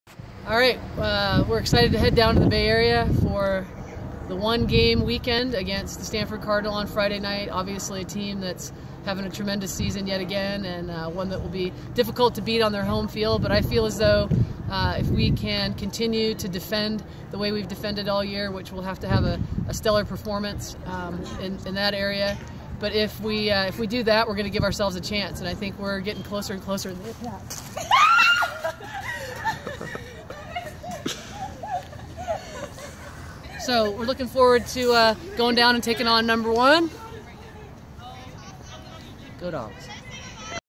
Women's Soccer: Lesle Gallimore Media Minute (10/11/17)
Coach Gallimore talks about the upcoming game against #1 Stanford.